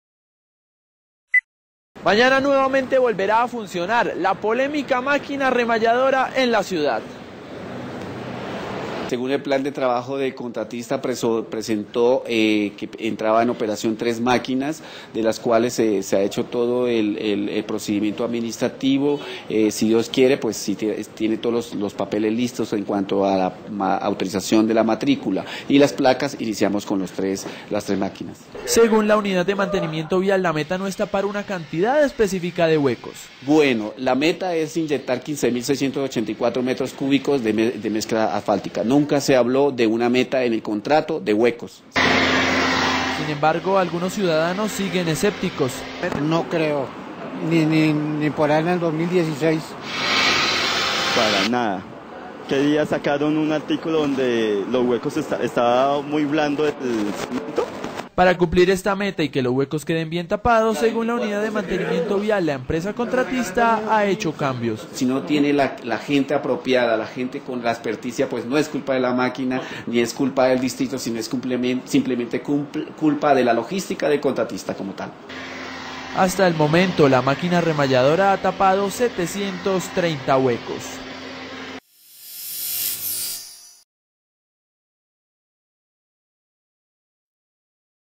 CityNoticias: Se reanuda funcionamiento de la máquina remalladora
Emisión: 5 de septiembre de 2014.

Se reanuda funcionamiento de la máquina remalladora.